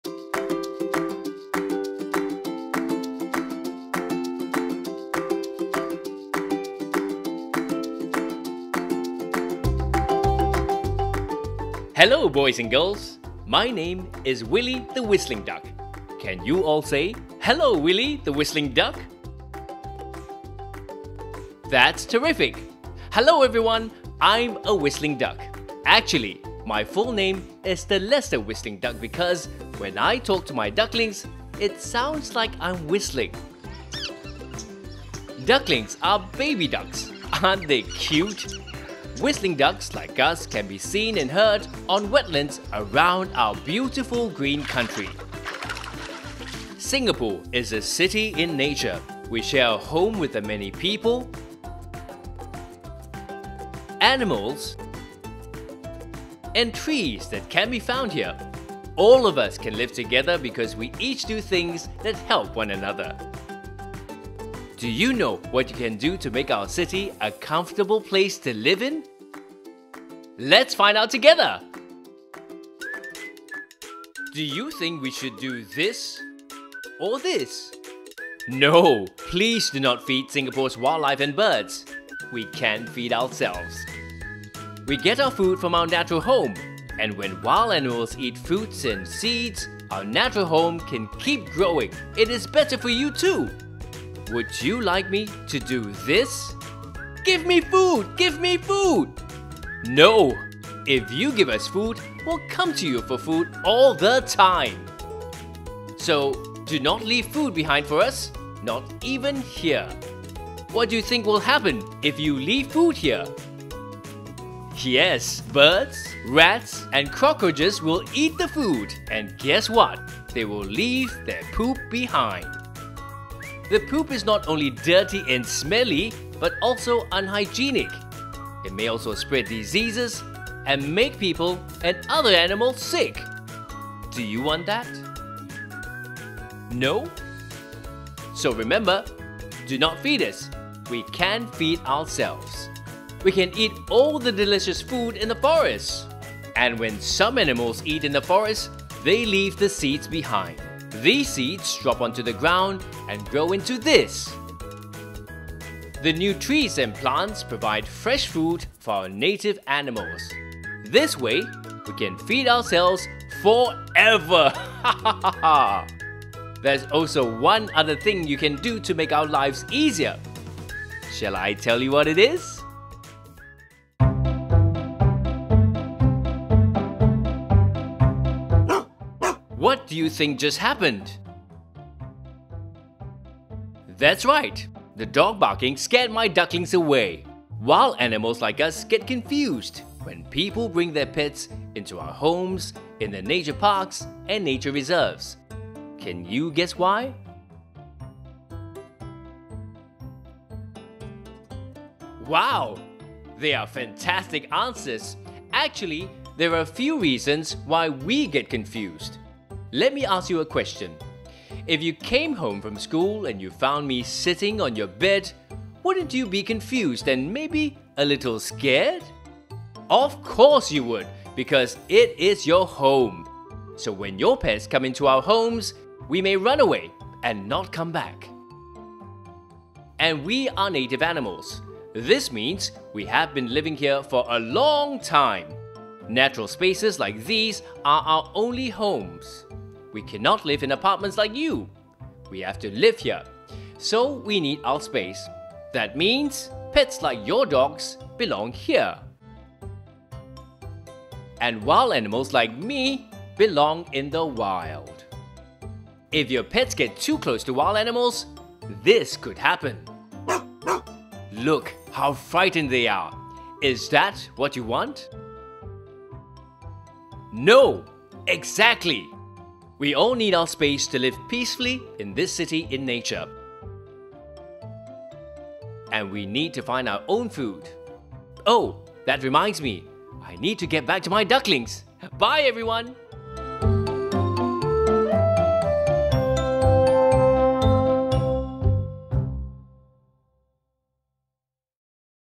Willie the Whistling Duck Explores our City in Nature
Follow Willie the Whistling Duck as he explores our City in Nature and discovers the people, native animals and pets that live here. There is much to learn about what we should and should not do around animals!

By the end of his journey, WIllie learns that all of us have to play our part to ensure our urban ecosystem remains liveable for all. What can we do to make our green city a more comfortable place to live in? What the video to find out.